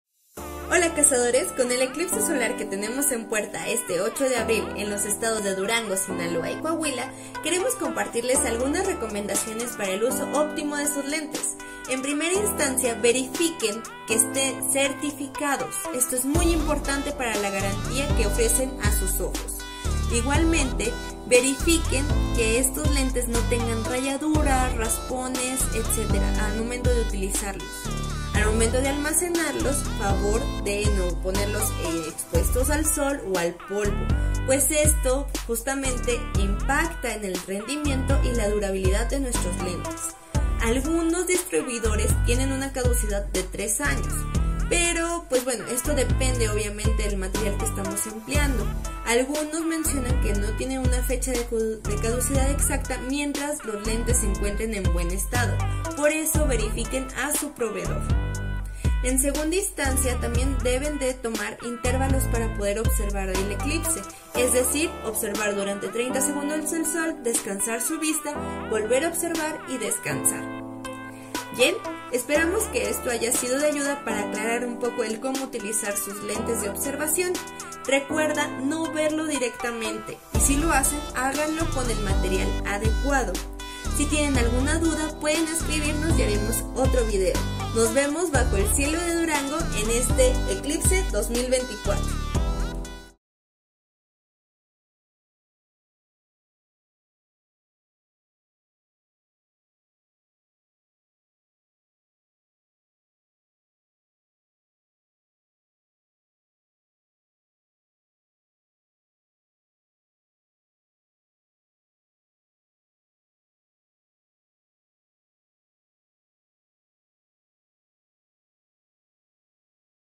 USO DE LENTES DE ECLIPSE SOLAR
✨ ¡Prepárate para el espectáculo del año! 🌒🔭 Descubre cómo disfrutar del eclipse solar del 8 de abril de 2024 de manera segura con los lentes🕶️ Recuerda: máximo 3 segundos de uso continuo, cuida tus lentes como tesoros y almacénalos donde no se dañen. 🌌
Escribenos tus dudas para los siguientes videos.
Para más información sobre cómo disfrutar del eclipse, visita el enlace en nuestra bio ¡Nos vemos bajo el cielo estrellado de Durango! ✨